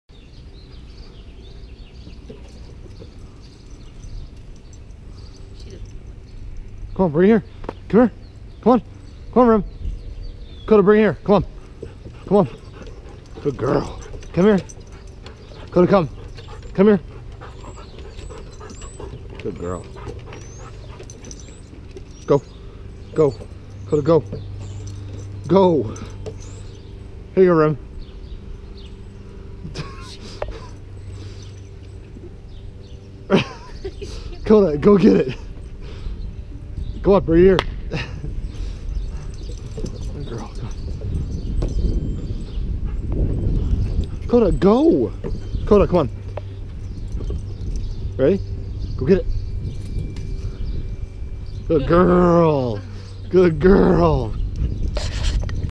Remi and Dakota playing Frisbee
My Vizsla (Dakota) and my Dad's GSP (Remi) playing in my Back yard. This is my first GoPro video.